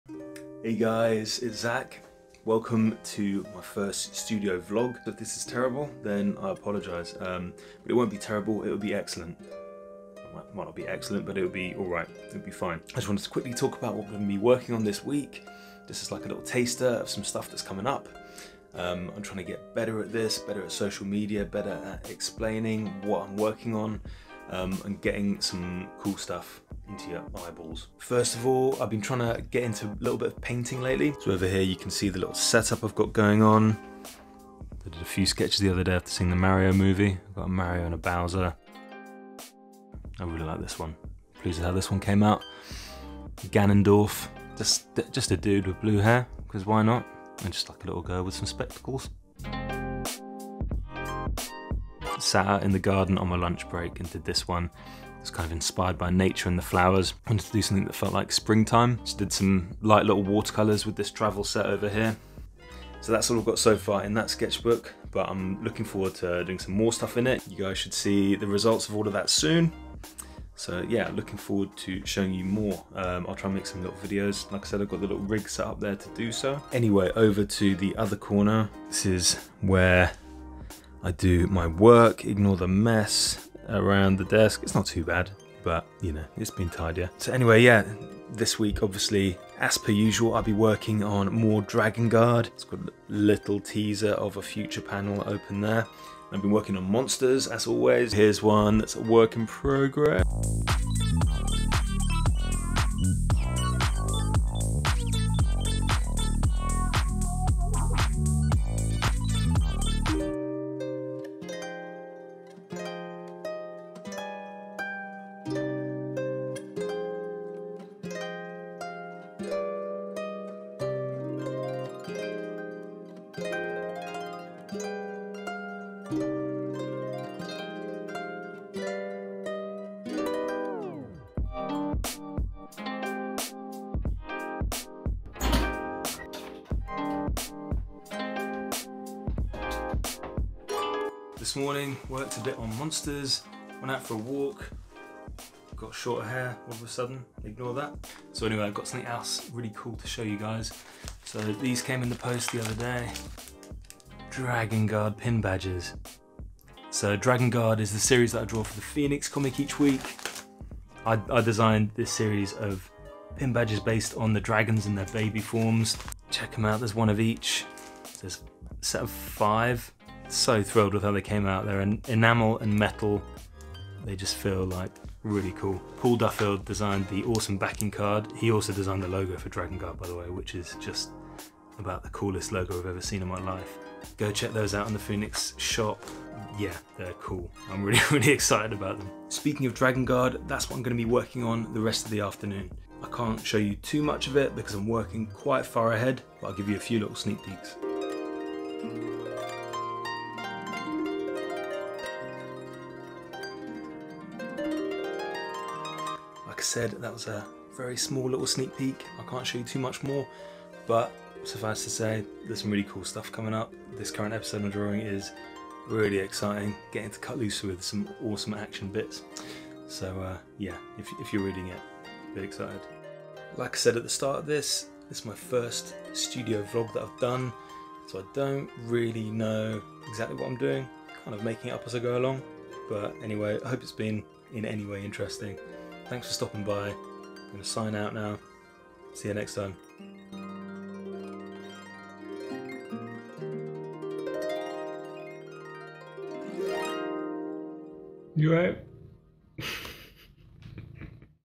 Studio Vlog 1- May 2023
In my first studio vlog I try to share a brief snapshot of my working day as a comic book artist.

This time I delve into my sketchbook, draw monsters, receive a special package and share a sneak peek at some upcoming Dragon Guard panels I'm working on!